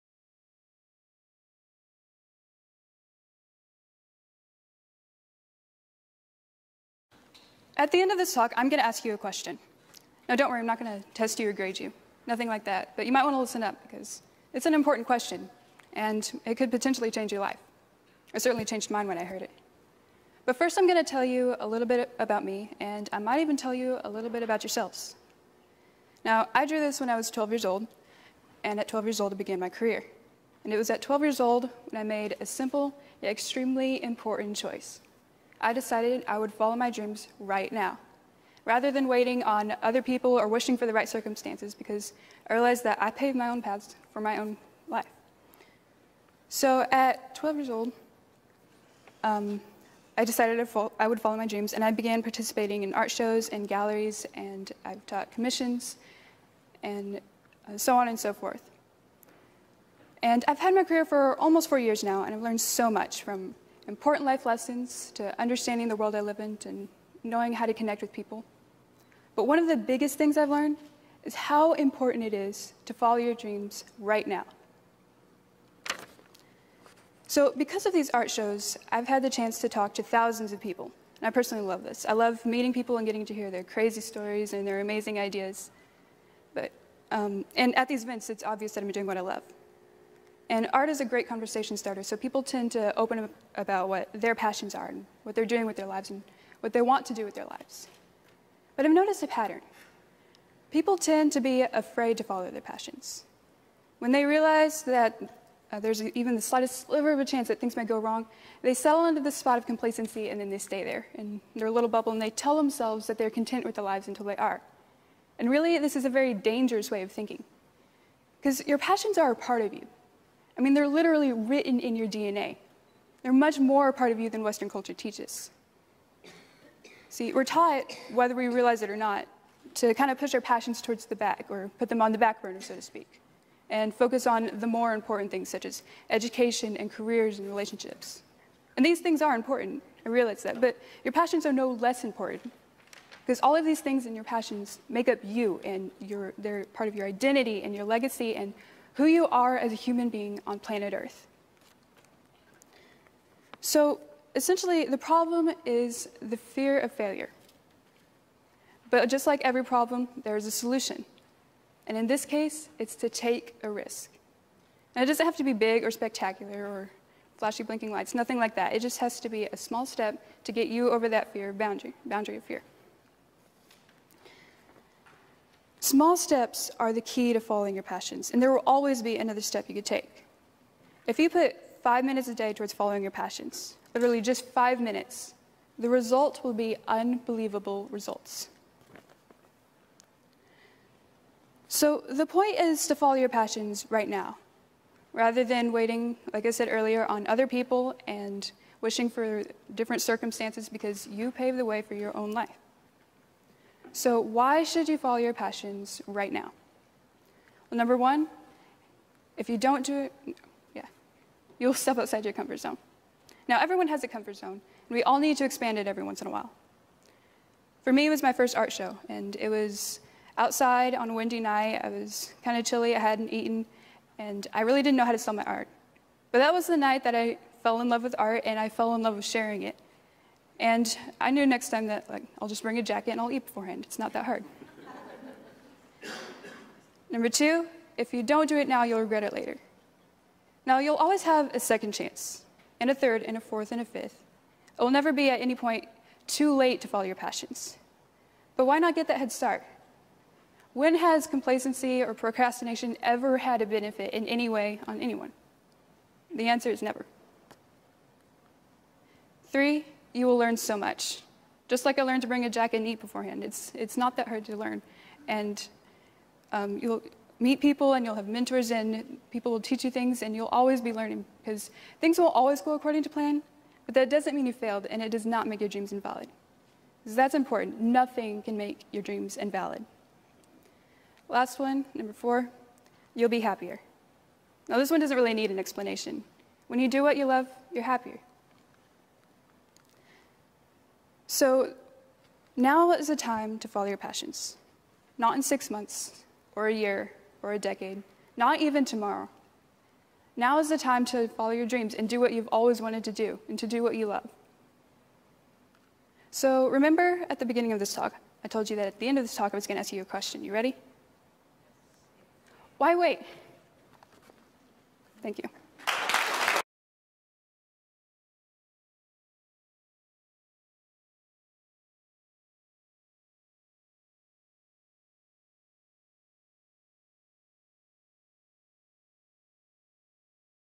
11 22 2015 Sadie Thompson Don't Wait
Sadie Thompson shares her wonderful art skill and shares the impact of striving for goal and dreams today. Don't Wait is about fighting procrastination and making dream become reality. In her TED Talk she offers a perspective that helps dreams become achievable vs. fade over time. Her TED Talk was at Trinity Valley School in Fort Worth, TX at TEDxYouth Fort Worth.